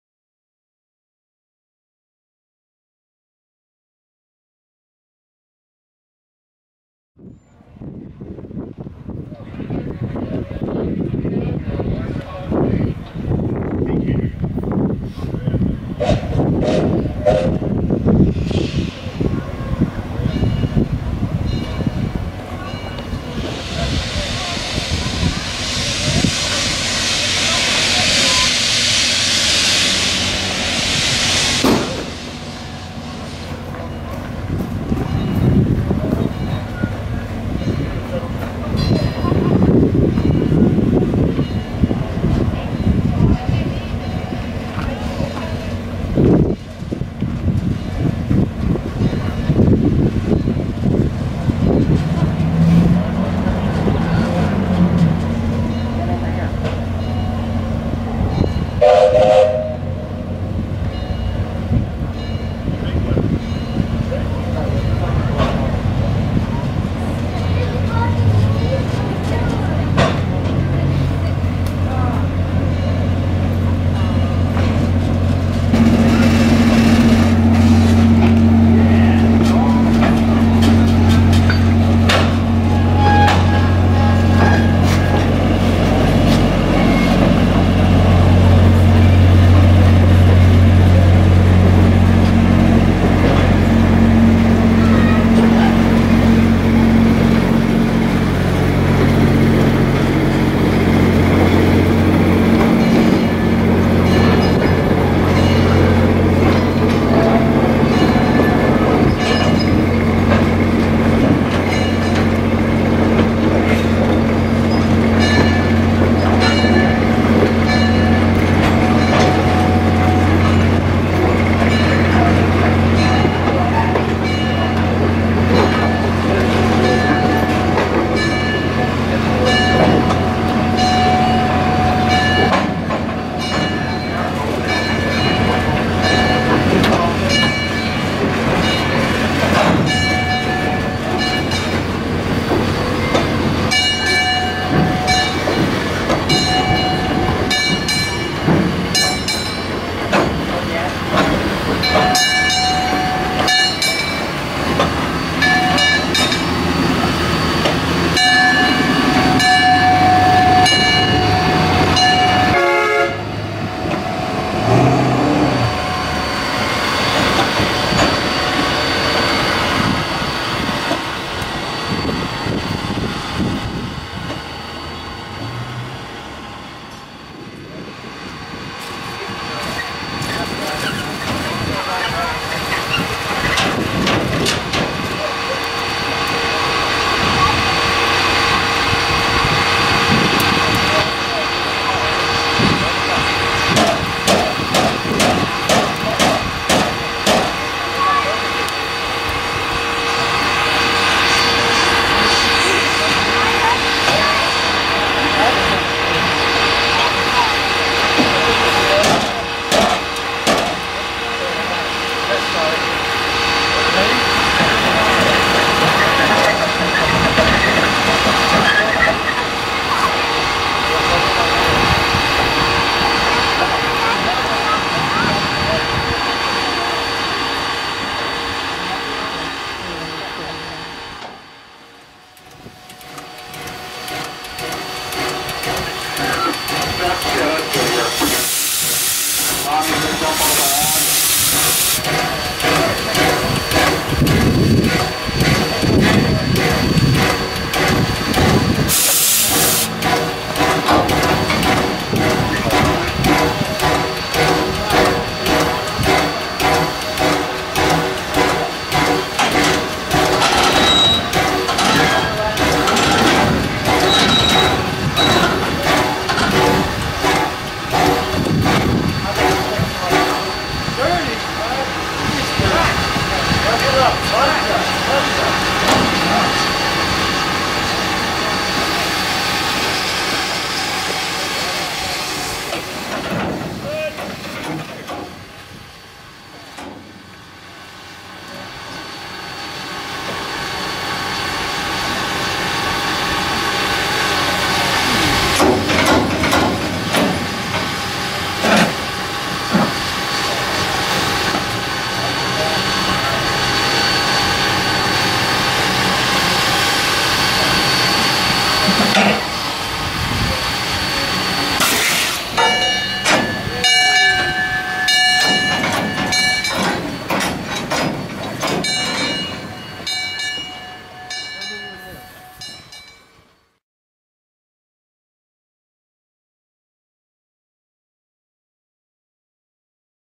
Vintage Steam Train - Post Excursion / Dumping Fire Box
The final run of the Lehigh Valley 0-6-0 Steamer as it wraps up its 2014 visit on the WSRY in Frederick County, Maryland.